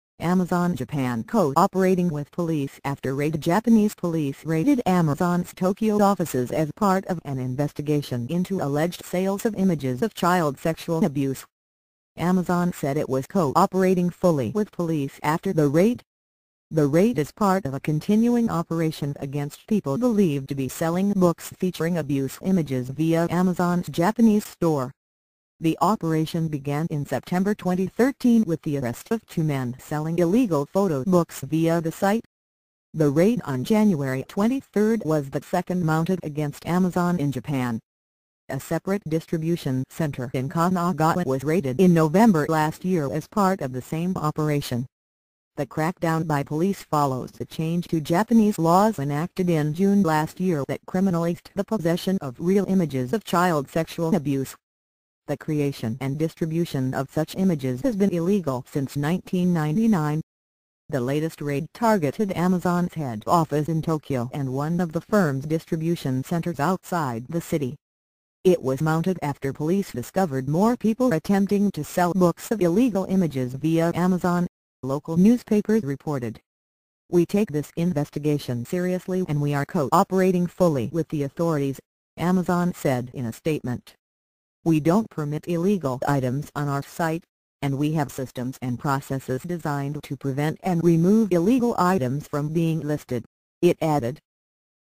Amazon Japan 'co operating' with police after raid : 24/7 News online
Amazon Japan 'co-operating' with police after raid

Japanese police raided Amazon's Tokyo offices as part of an investigation into alleged sales of images of child sexual abuse.

Amazon said it was "co-operating fully" with police after the raid.

The raid is part of a continuing operation against people believed to be selling books featuring abuse images via Amazon's Japanese store.

The operation began in September 2013 with the arrest of two men selling illegal photo books via the site.

The raid on 23 January was the second mounted against Amazon in Japan. A separate distribution centre in Kanagawa was raided in November last year as part of the same operation.

The crackdown by police follows a change to Japanese laws enacted in June last year that criminalised the possession of real images of child sexual abuse. The creation and distribution of such images has been illegal since 1999.

The latest raid targeted Amazon's head office in Tokyo and one of the firm's distribution centres outside the city. It was mounted after police discovered more people attempting to sell books of illegal images via Amazon, local newspapers reported.

"We take this investigation seriously and we are co-operating fully with the authorities," Amazon said in a statement.

"We don't permit illegal items on our site, and we have systems and processes designed to prevent and remove illegal items from being listed," it added.